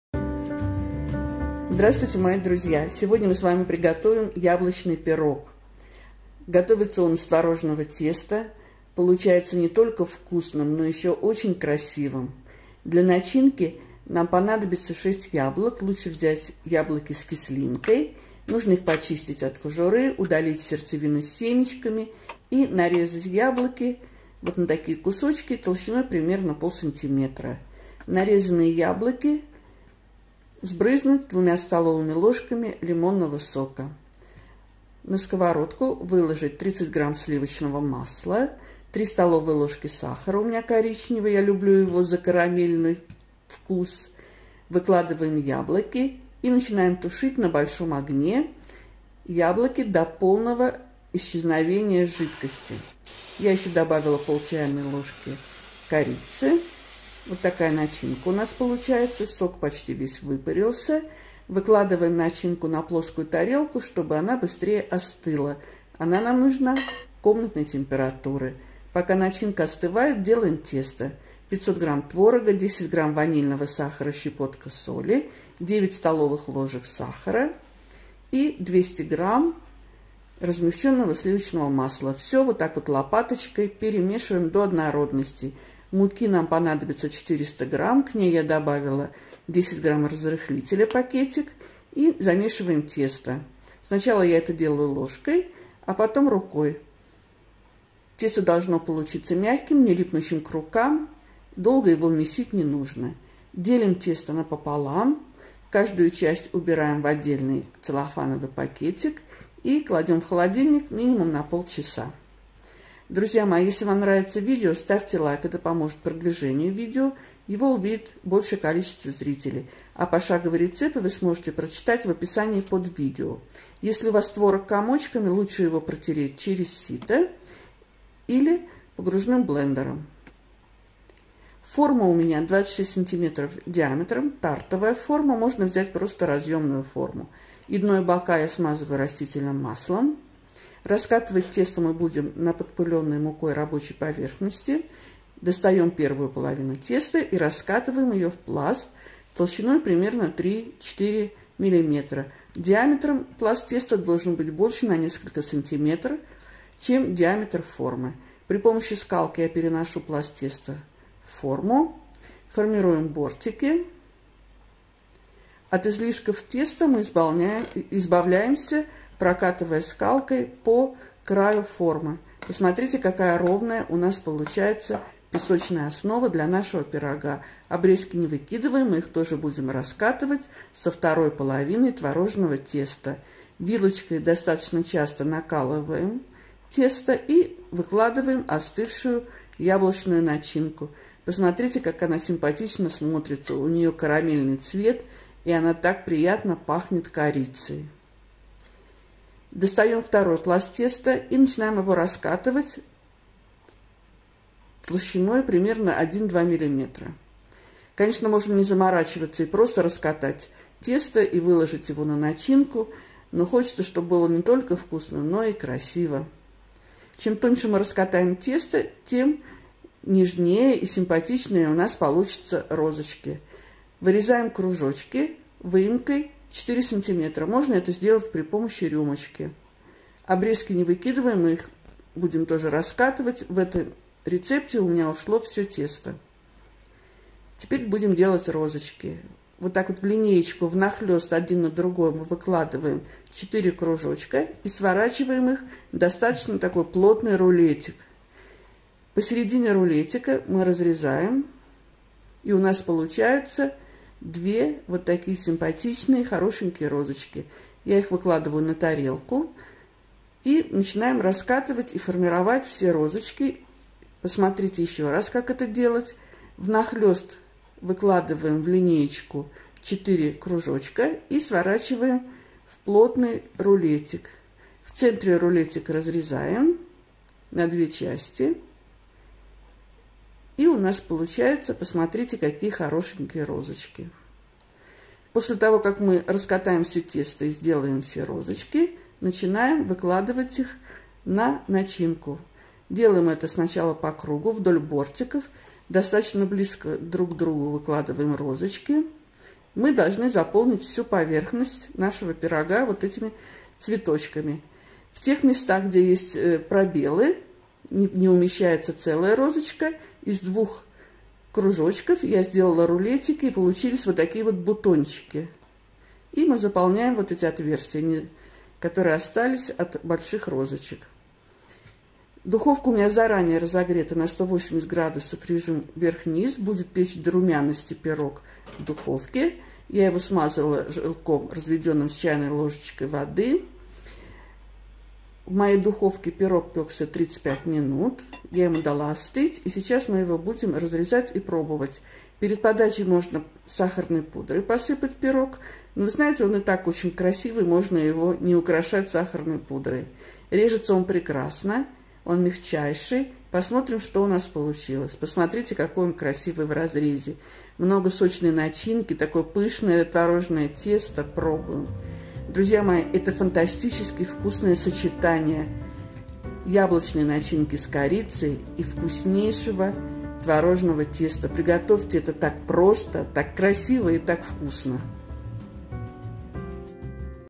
ЯБЛОЧНЫЙ ПИРОГ ТВОРОЖНЫЕ РОЗЫ! Это так просто, так вкусно и так красиво!
Вкуснейшее творожное тесто, сочная яблочная начинка с корицей! Чаепитие с этим пирогом будет фантастически вкусным!
Форма 26 см
Рецепт:
Тесто:
500 гр творог
200 гр слив.масло
160 гр сахар ( 9 ст.л )
400 гр мука
10 гр разрыхлитель
10 гр ванильный сахар
Соль щепотка
Начинка :
6 шт яблоки
80 гр сахар ( 3 ст.л )
2 ст.л лимонный сок
30 гр слив.масло
0,5 ч.л корица
Для смазки 1 желток и 1 ч.л молока или воды.
1. Яблоки очистить от кожуры, удалить сердцевину с семечками, нарезать на кубики и полить лимонным соком. В сковородке растопить слив.масло, добавить сахар, яблоки и тушить все на большом огне до полного испарения жидкости, добавить корицу.
2. В миске смешать до однородности творог, размягченное слив.масло, сахар, ванильный сахар, соль. Затем всыпать муку, разрыхлитель и замесить тесто. Долго месить не нужно, просто собрать тесто в шар. Оно должно быть мягким и не липнуть к рукам. Резделить тесто на 2 части и убрать в холодильник на 30 минут.
3. Раскатать первую половину теста в пласт толщиной 3 мм, диаметром на 3-4 см больше чем форма. Выложить тесто в смазанную раст.маслом форму, обрезать излишки теста и наколоть часто вилкой.
Выложить на тесто ровным слоем остывшую начинку.
4. Вторую половину теста раскатать в пласт толщиной 1-2 мм. Вырезать рюмкой или выемкой 4 см кружки. Сформировать розочки как показано в видео. Выложить розочки на начинку. Смазать желтком смешанным с молоком верх пирога. Выпекать до румяности 30-35 минут в заранее разогретой на 180 градусов режим верх - низ духовке. Перед подачей посыпать остывший пирог сахарной пудрой. Приятного чаепития!